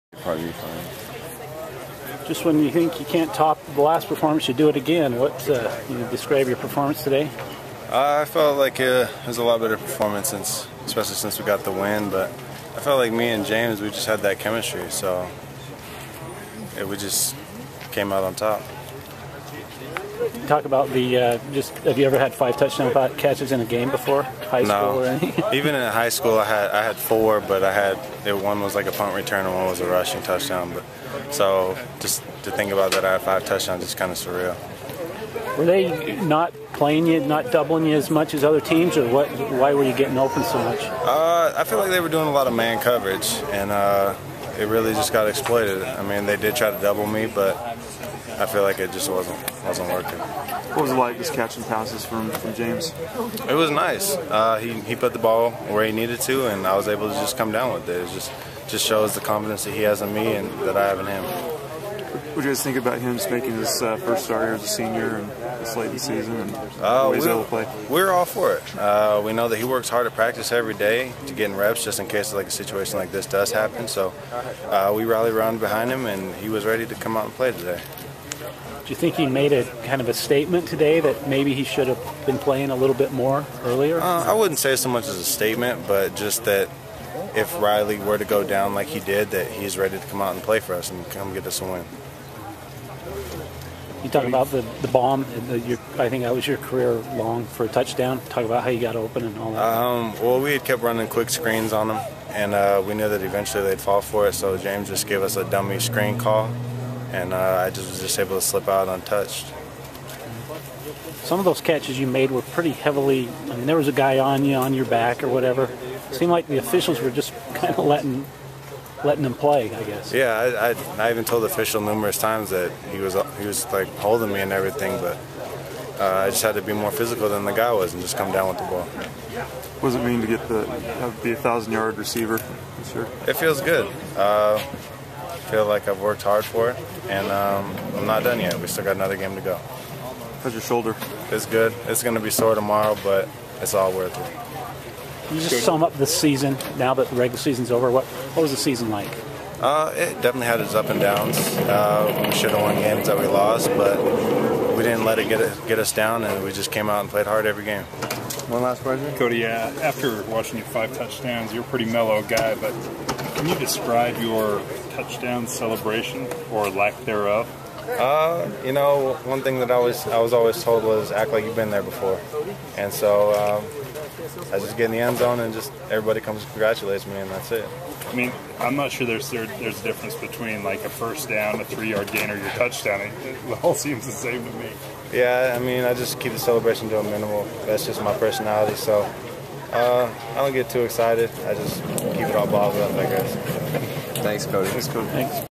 BYU receiver Cody Hoffman after five touchdown catches
BYU receiver Cody Hoffman talks about his record-setting day after the Cougars defeated New Mexico State 50-14 on Saturday in Las Cruces.